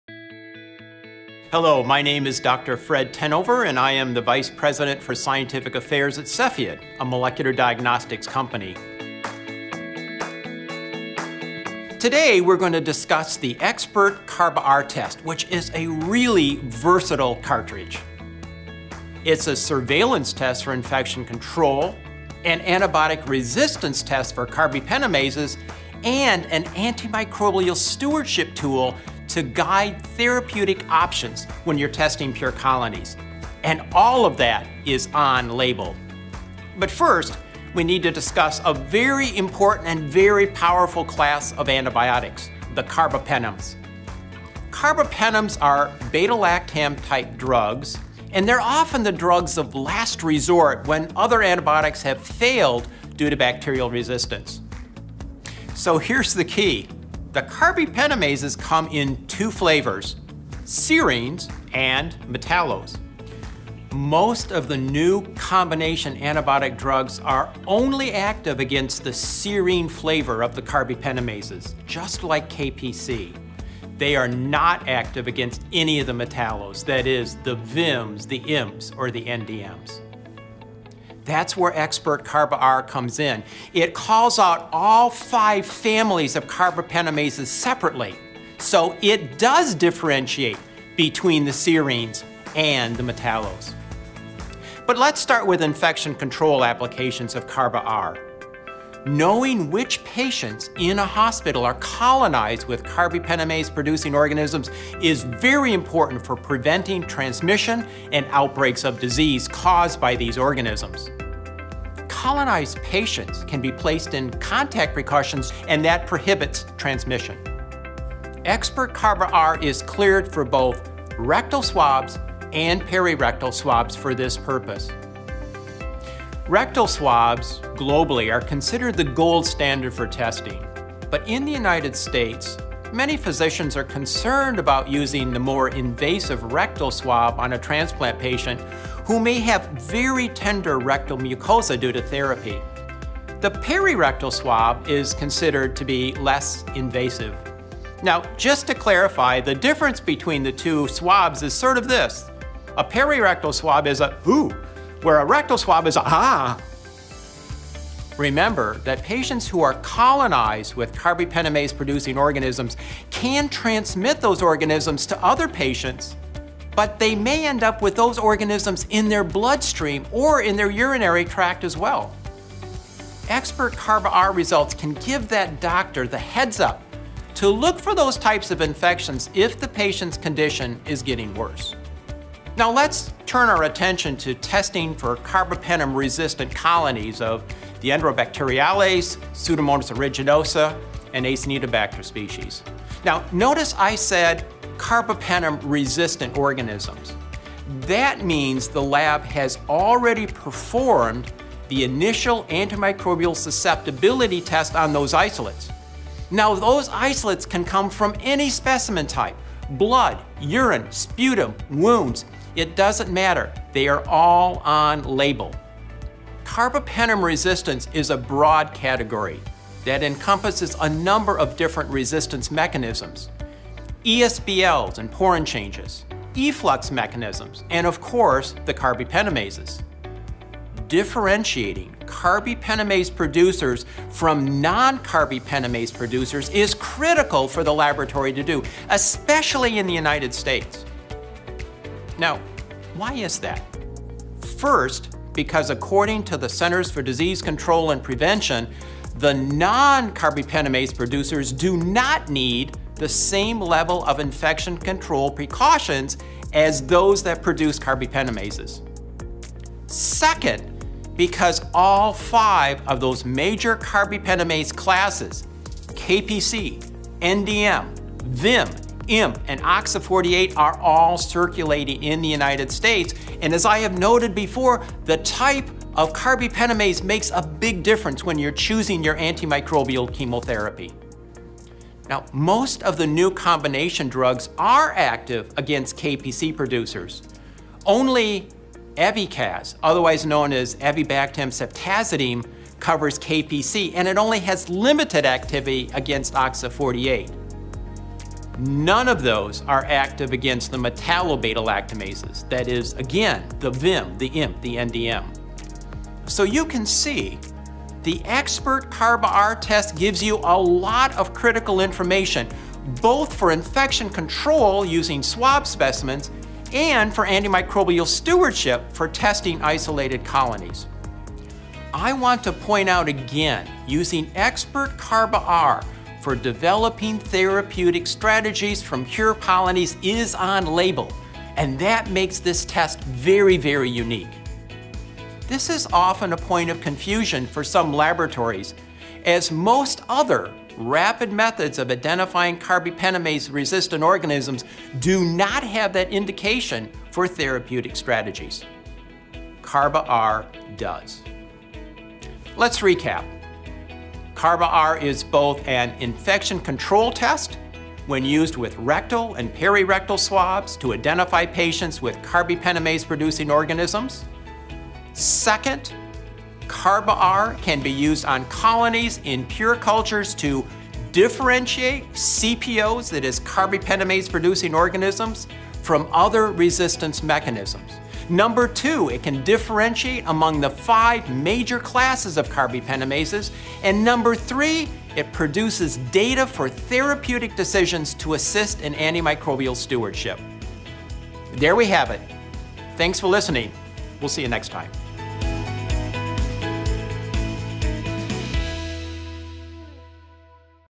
Dr. Tenover (retd) Discusses The Benefits of Cepheid's Xpert Carba-R Test
Dr. Fred Tenover (retd) discusses the Benefits of Cepheid's Xpert(R) Carba-R test.